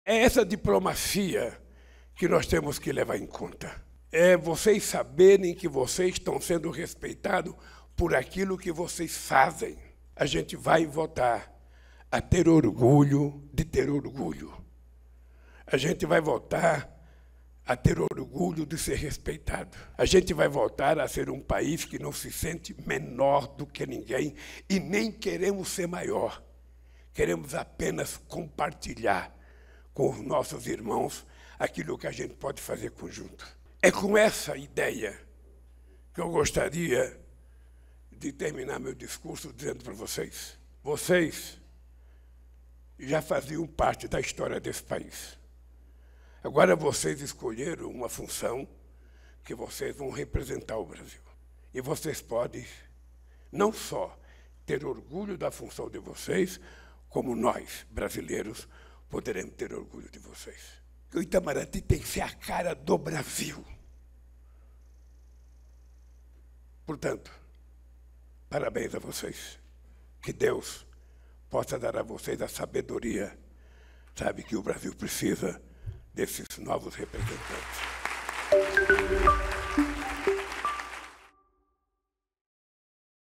Itamaraty com a cara do Brasil 🇧🇷
Ao representar o Brasil, queremos resgatar o orgulho de ser um país que se sente valorizado e valoriza seus irmãos. Juntos, compartilharemos conquistas e construiremos uma história de respeito mútuo. O Itamaraty será a cara do Brasil.
🎥 Audiovisual/ PR